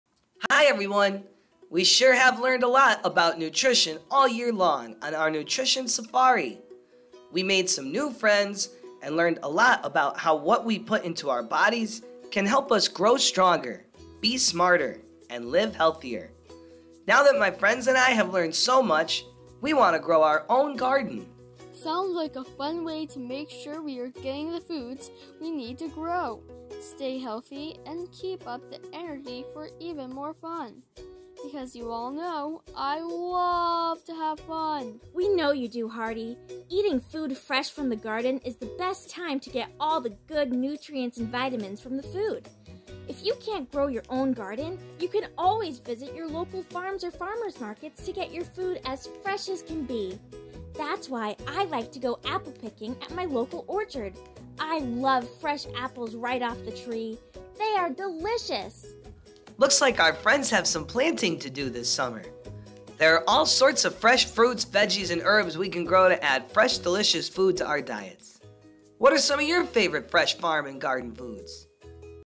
Whitsons Nutrition Safari - June - Luke the Lion and Friends Fresh From the Garden
Join the Safari friends as we learn what foods grow locally in our community. This usually includes fresh fruits and vegetables and herbs, and we can find them at farmer’s markets, local farms and even our grocery stores in special sections. The benefit of eating locally grown foods is that they are straight from the farm full of all their natural nutrients and flavors. It’s also a nice way to support our farmers and help the environment at the same time.